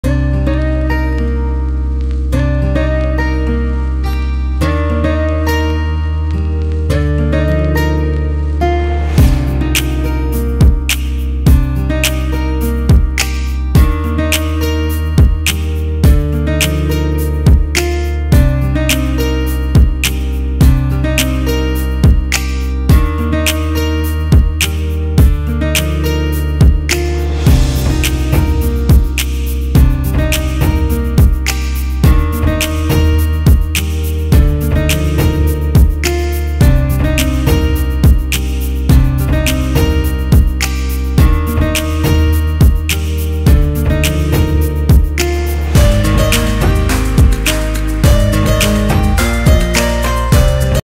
ICONIC London: Dolphin Skin with Mena Adubea
Video credit: @MENA_ADUBEA ❤️

Get glowing, lit-from-within Dolphin Skin in 4 easy steps with our ICONIC bestsellers:

1. Sheer Blush in Rose Riot: Gel-to-watercolour formula that creates a veil of glowing colour.
2. Illuminator in Original: Tap onto high points of the face for a blinding highlight.
3. Lustre Lip Oil in She's A Peach: Roll across your lips for hydration and intense gloss.
4. Prep-Set-Glow in Original: Spritz over your finished look for an final shimmering flourish.